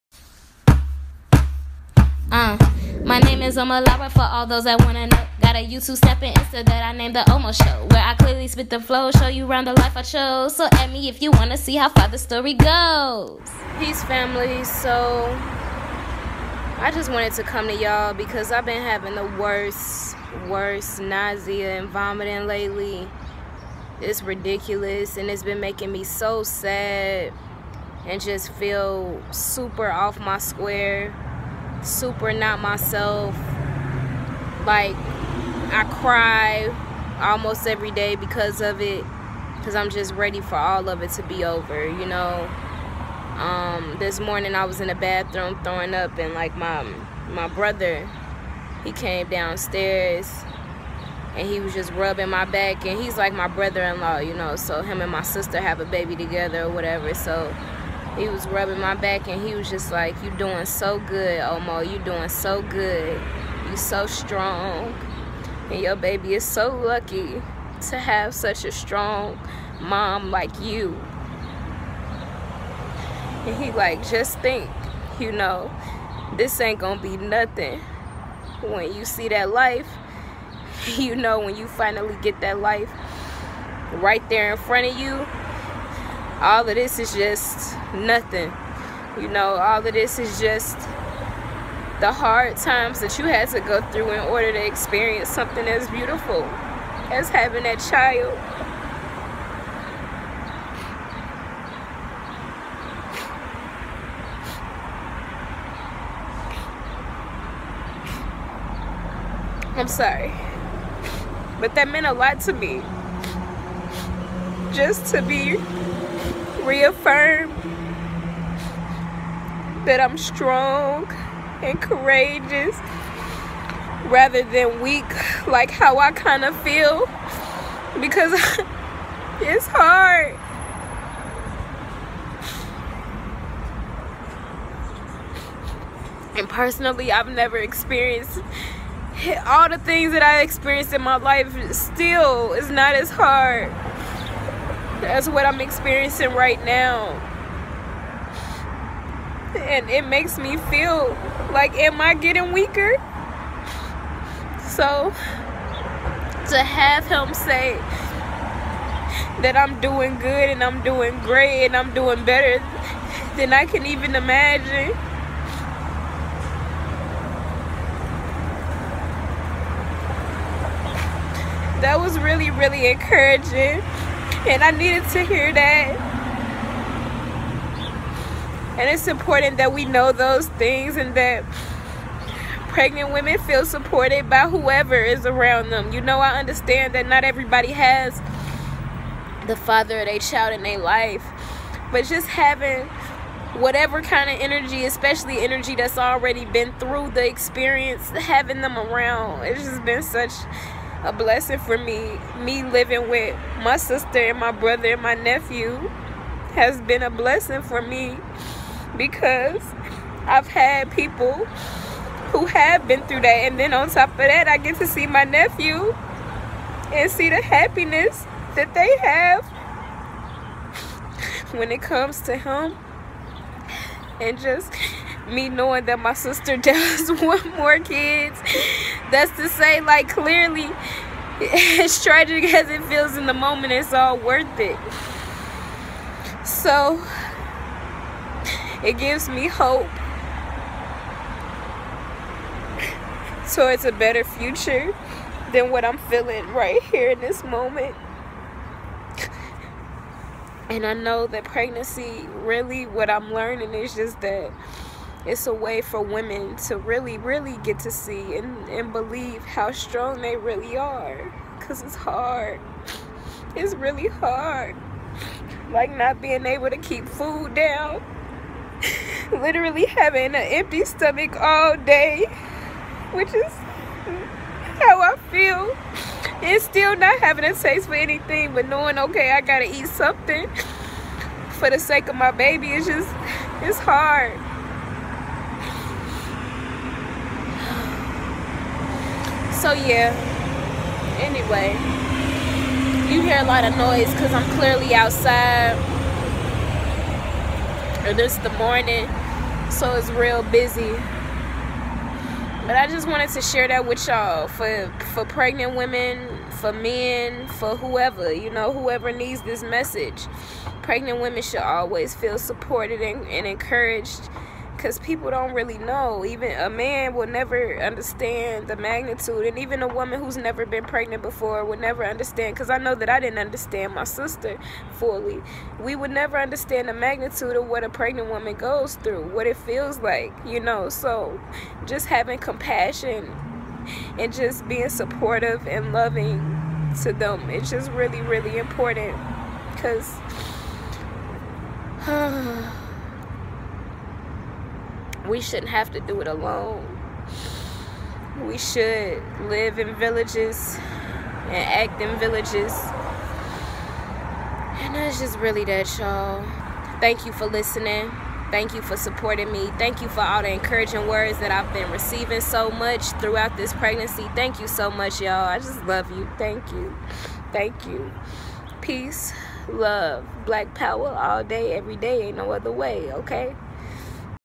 Being Pregnant is HARD
Being pregnant has been the hardest thing I've ever done in my life thus far - and I've been through a lot! Here is an emotional video on how pregnancy has been for me (HARD!), and how the people around pregnant women could be supporting her.

Peace, love, and Black Power
Omo Nyame (IG: @OmoNyame)

wanna hear some dope music?
Apple Music/Spotify : search "Omo Nyame"